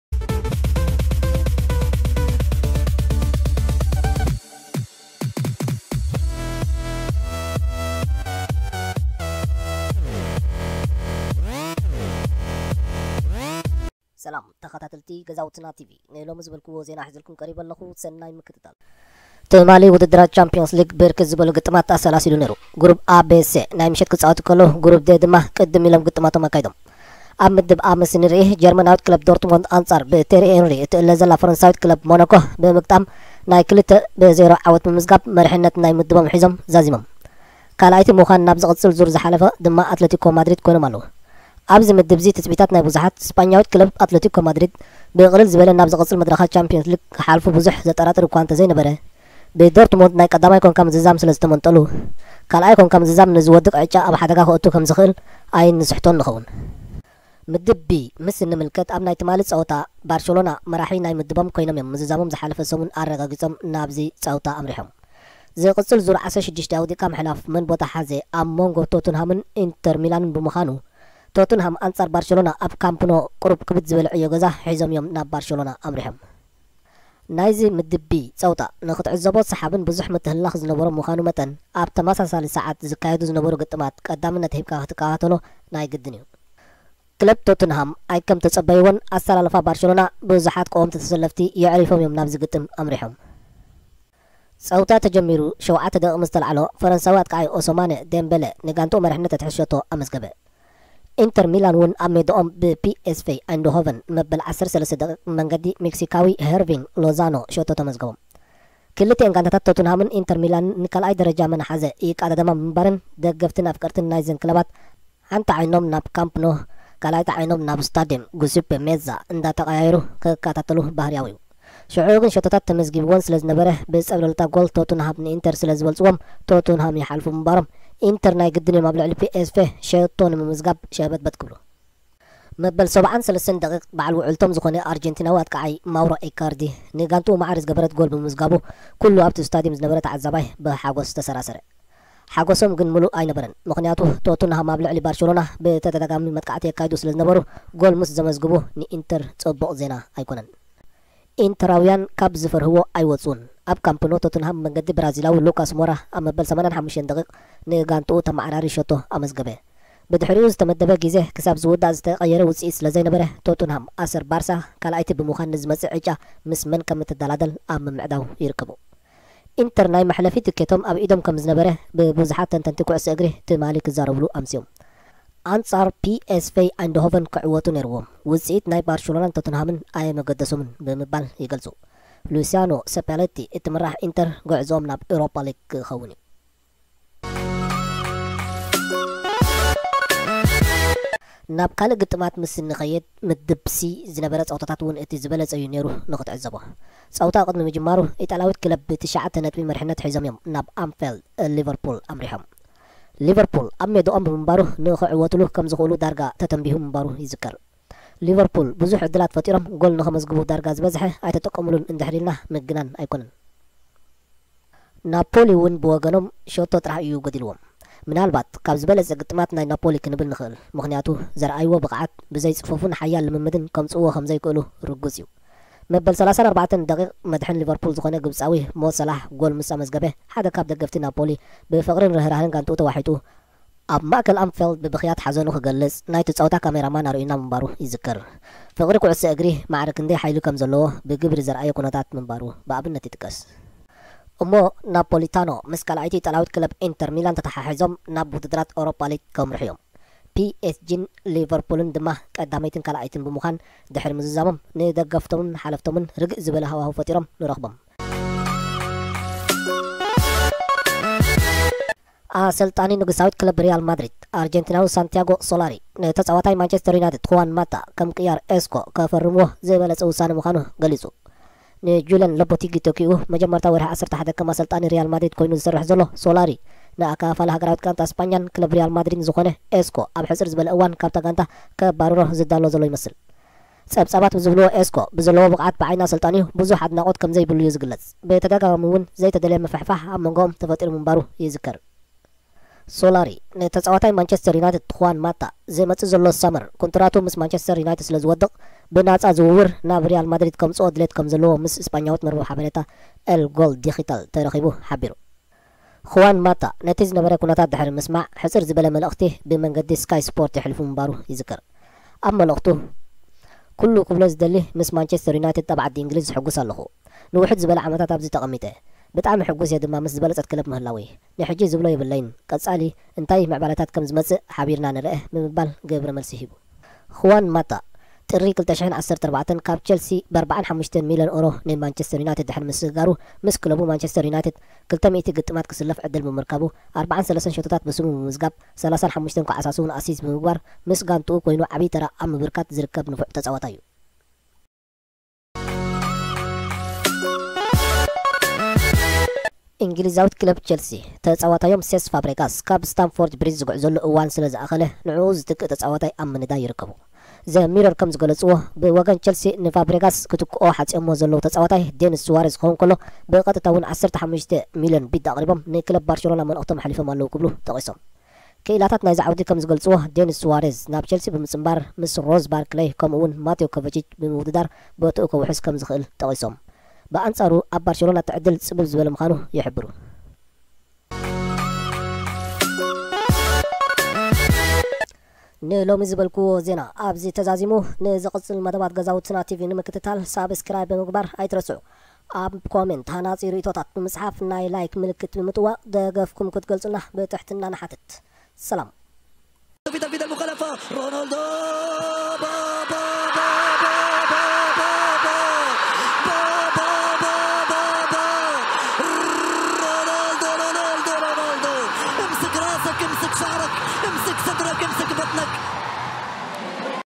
12-12-2018#ጸብጻብ ቻምፕዮንስ ሊግ ፨ፋብረጋስ ምስ ቸልሲ ዝወድኤ ይመስል፨ኢስኮ ምስ ኣሰልጣኒኡ ኣይተሰማምዐን፨
Gezawtena tv gives a short football news and educational.ሰላም ተከታተልቲ ገዛውትና TV ኣብ ቀረባ እዋን ብስፍሕ ዝበለ ማሃርን ጠቃምን ዝበልናዮም መደባት ሒዝናልኩም ከም እንቀርብ በዚ ኣጋጣሚ ክንሕብረኩም ንደሊ፦ንዓና ንሞራል